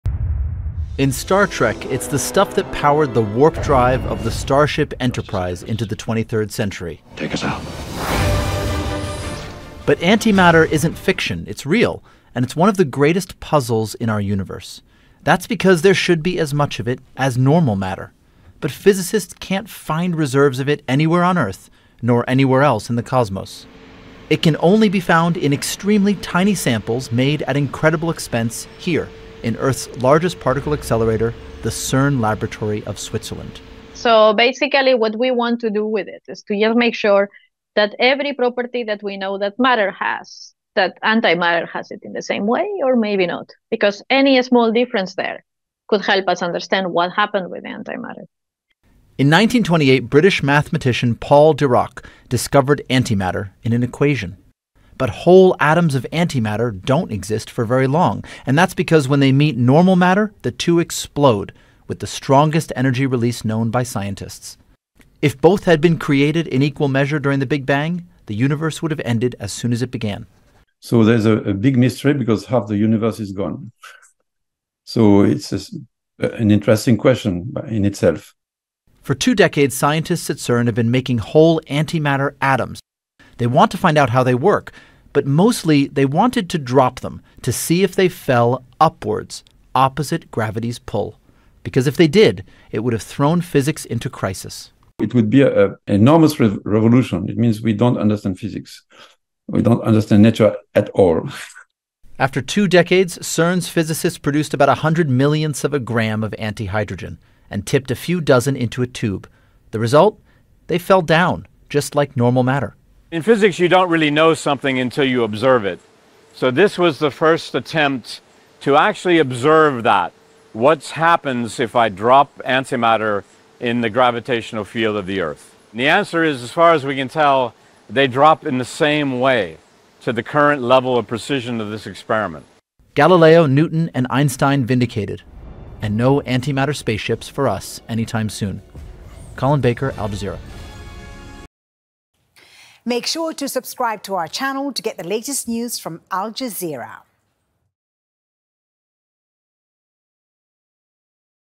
Gravity test: Antimatter falls down, but where did it all go?
One of the great mysteries of physics has been solved: How does antimatter … fall?
It is not a question that keeps most people awake at night, but some physicists have been waiting for its answer for years.
Scientists at CERN, the world’s largest particle accelerator in Switzerland, announced Wednesday that a pathbreaking experiment had confirmed that antimatter falls down with gravity — just like everything else. But that only leaves scientists asking more questions about this curious material.

Al Jazeera’s Colin Baker explains.